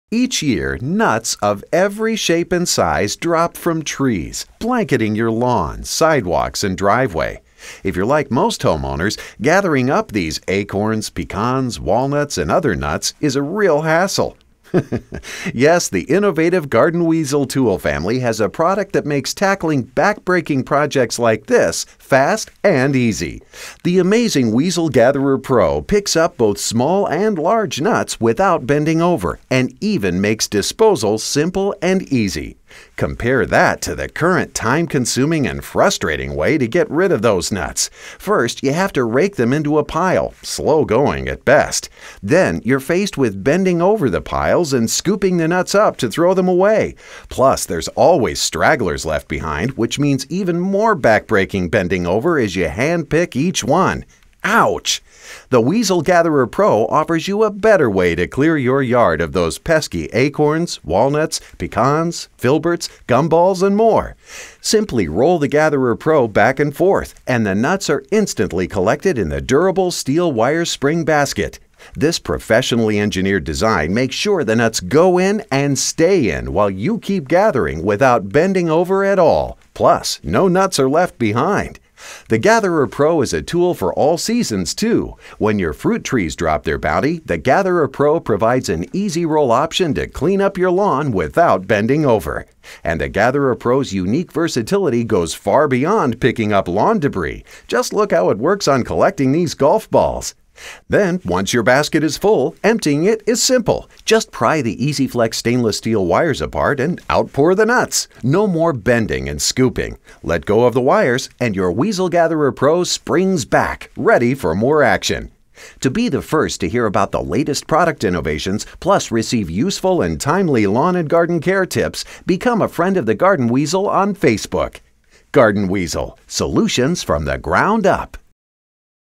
Garden Weasel Nut Gatherer Pro
Picks up acorns, walnuts, butternuts, hickory nuts, chestnuts, filberts, gum balls, seeds, and more.  The Weasel Nut Gatherer Pro is an easy to use rolling pick up tool for nuts and more.  No bending over.  Saves time. Also picks up fruit and small balls.

For more tips on our tools and putting them to work for you in your lawns and gardens, join us on Facebook